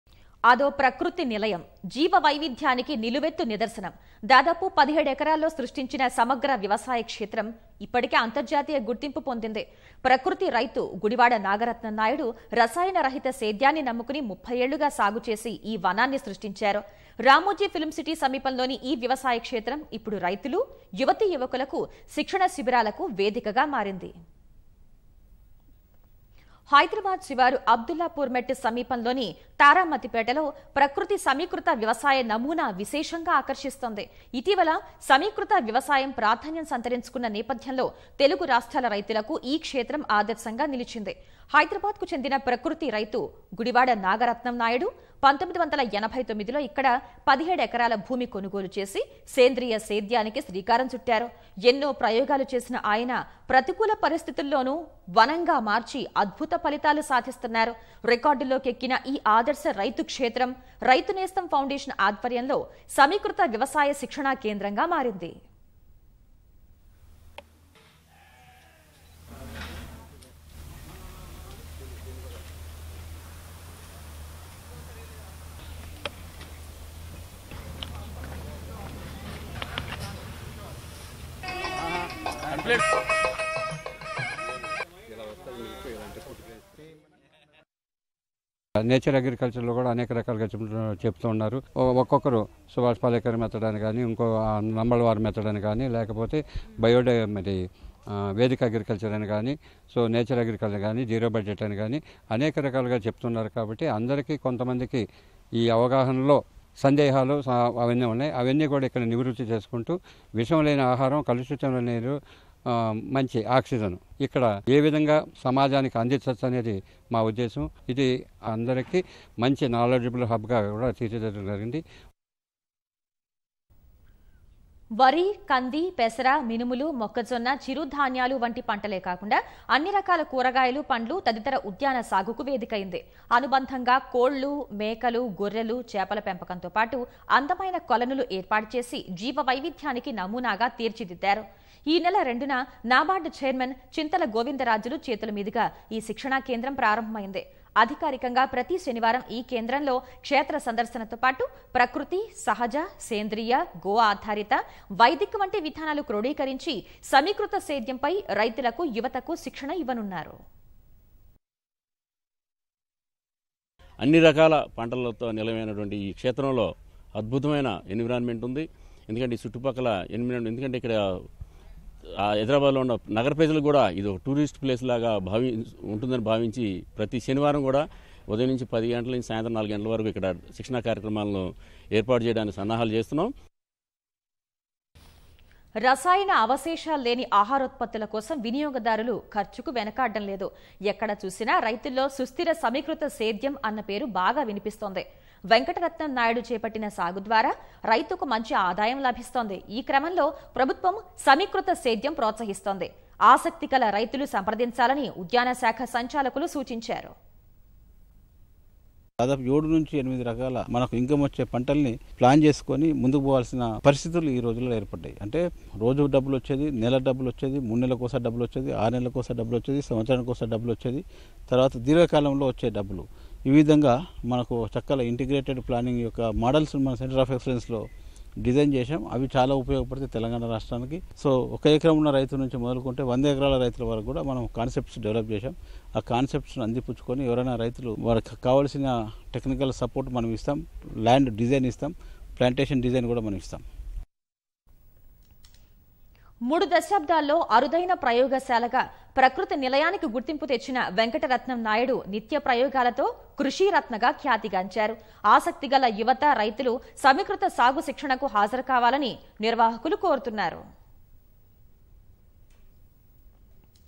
సేంద్రియ వ్యవసాయ క్షేత్రంపై కథనం | Raithu Nestham Foundation's Training Center
అదో ప్రకృతి నిలయం... జీవవైవిధ్యానికి నిలువెత్తు నిదర్శనం. దాదాపు 17 ఎకరాల్లో సృష్టించిన సమగ్ర వ్యవసాయక్షేత్రం... ఇప్పటికే అంతర్జాతీయ గుర్తింపు పొందింది. ప్రకృతి రైతు గుడివాడ నాగరత్నం నాయుడు... రసాయన రహిత సేద్యాన్ని నమ్ముకుని  30ఏళ్లుగా సాగు చేసి ఈ వనాన్ని సృష్టించారు. రామోజీ ఫిల్మ్ సిటీ సమీపంలోని ఈ వ్యవసాయ క్షేత్రం... ఇప్పుడు రైతులు, యువతీయువకులకు శిక్షణ శిబిరాలకు వేదికగా మారింది.